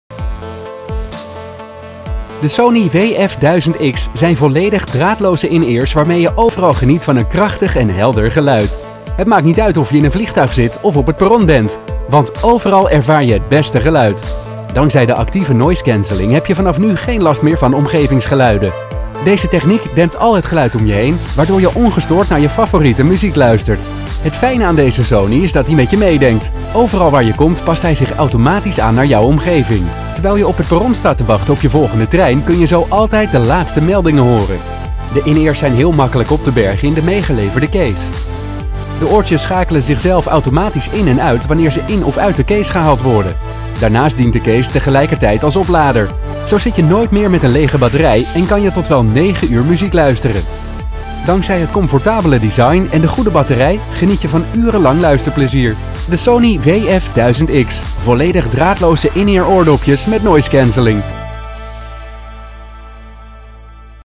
Sony WF1000X in-ear oordopjes - Productvideo - MediaMarkt
In-ear-oordopjes zonder draad, zonder ruis met Noise Cancelling die zich aanpast naar je omgeving. Het kan met de

Het maakt niet uit waar je bent. Je ervaart overal het beste geluid met de Sony WF-1000X. Je hebt geen last meer van omgevingsgeluiden dankzij de actieve Noise Cancelling, zo geniet je altijd ongestoord van je favoriete muziek. Dankzij Smart Listening-functies wordt het geluid automatisch naar jouw omgeving aangepast.

Daarnaast gaat de accu van deze stijlvolle in-ears tot wel 9 uur lang mee en via de meegeleverde case laad je de oordopjes binnen 2 uur weer op.

Eigenschappen:
Draagwijze: In-ear
Noise Cancelling: Ja
Bluetooth: Ja
NFC: Ja
Gebruiksduur: 9 uur
Oplaadtijd: 2 uur
Geïntegreerde microfoon: Ja

Verpakkingsinhoud:
Oplaadstation, USB-kabel, 4x dopjes, vormondersteuners, comfortoordopjes